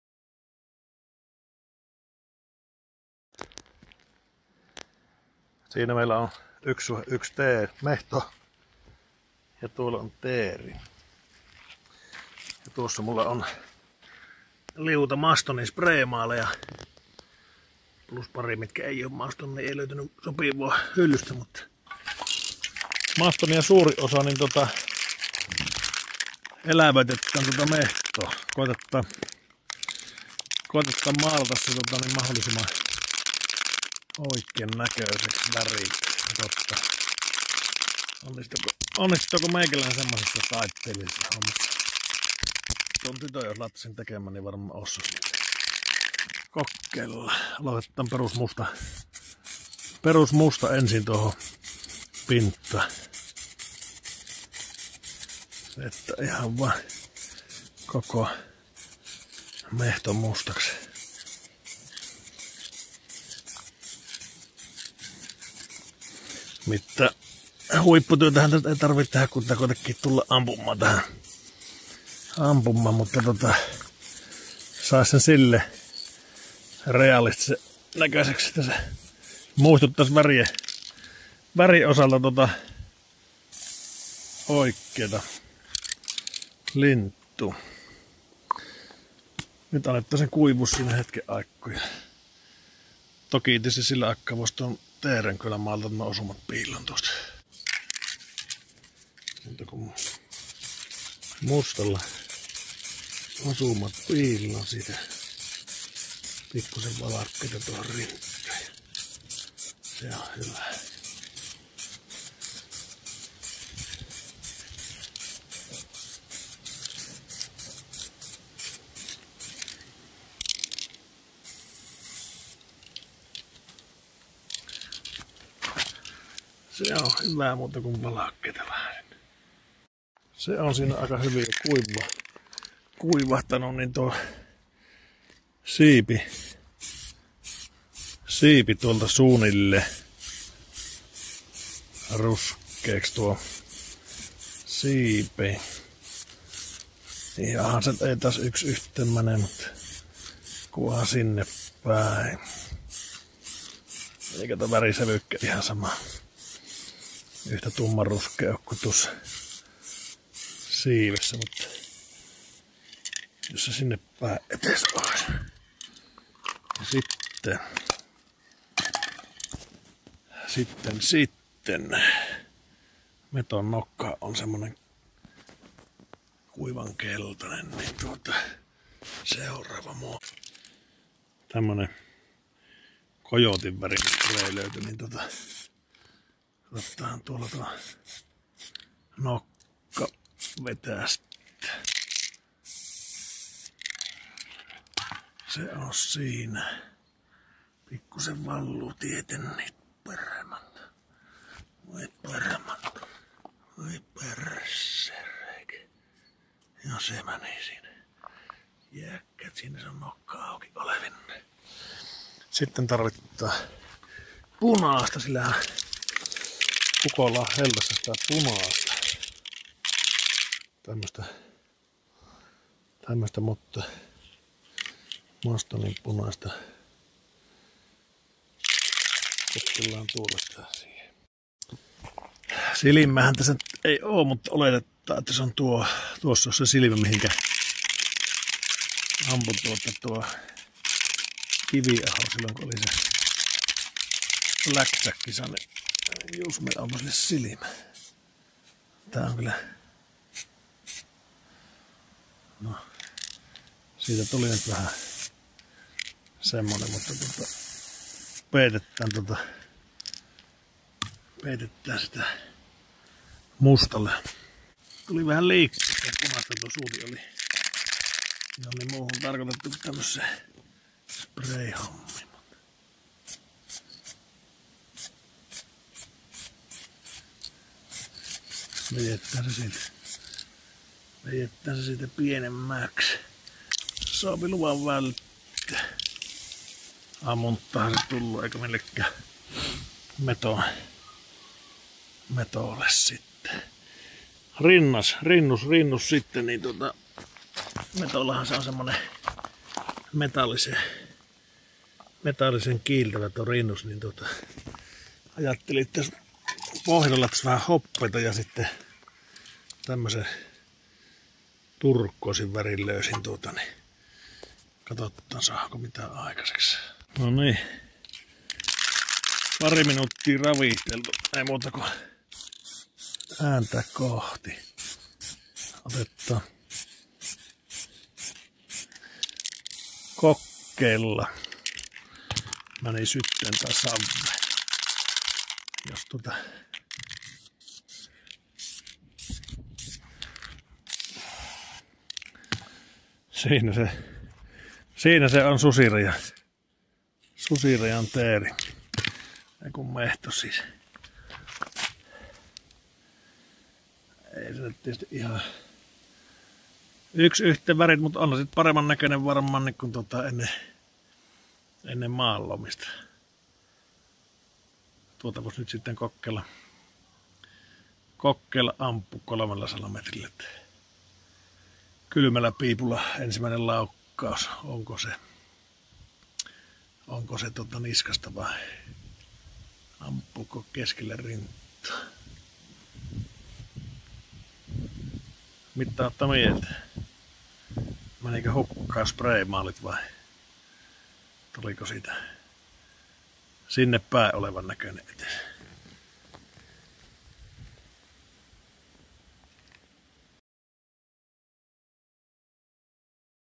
Maalataan Maston maaleilla metso ja teeri maalitaulut minttiin.
Maalasin spray maaleilla Hardox 500 sarjan metso ja teeri maalitaulut minttiin, maaleina käytin
Maaleja, ne on Suomessa tehty 🇫🇮💪🏻🇫🇮